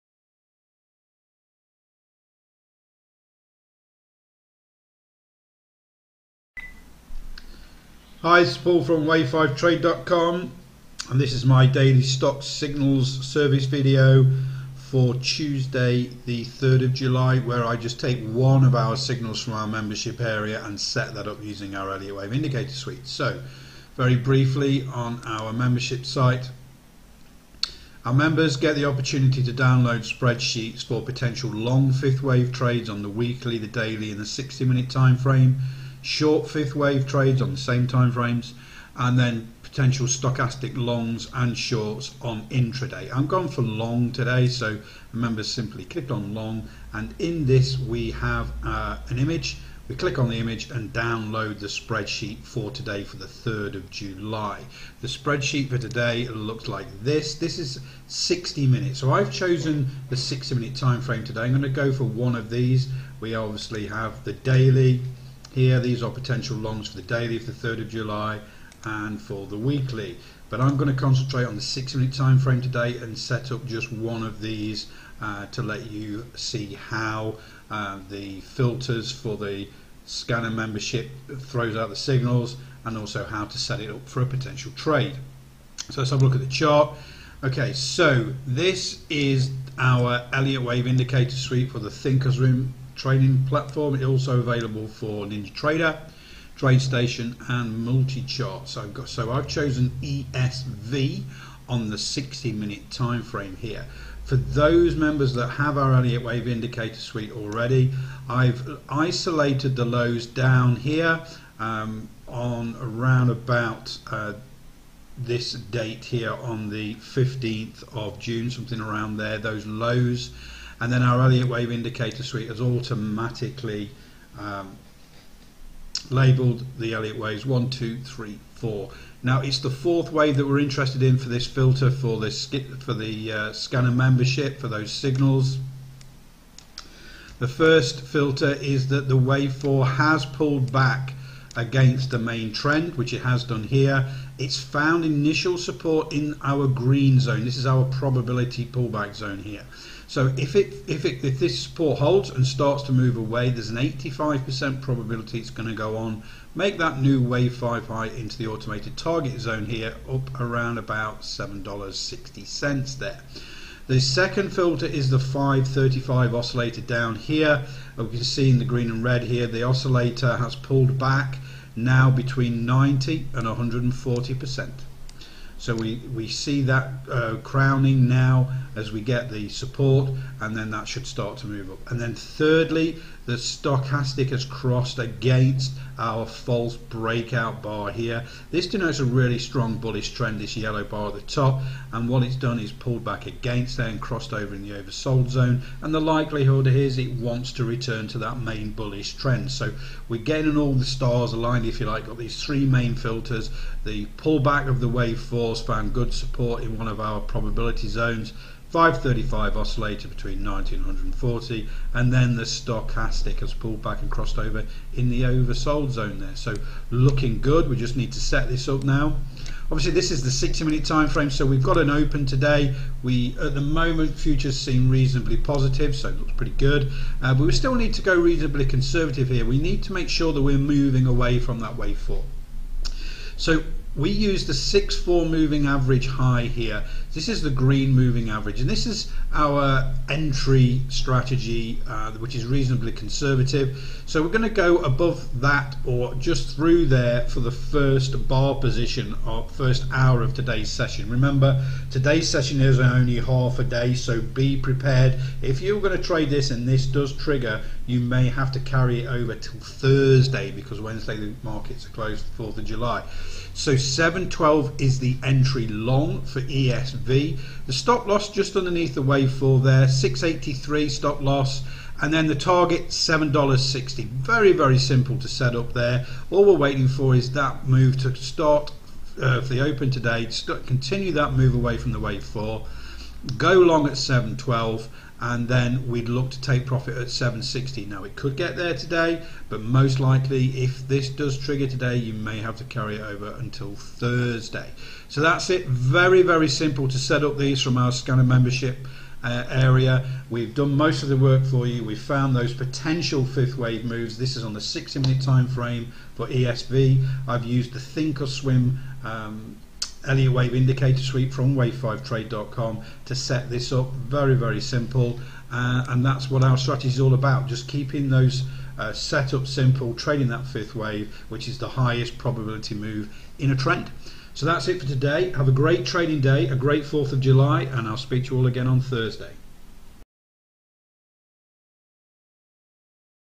Wave5trade Stocks Signals Video for potential Long on ESV
Watch the Stocks Swing Trading Setup video for a potential long trade on ESV using the Elliott Wave Indicator Suite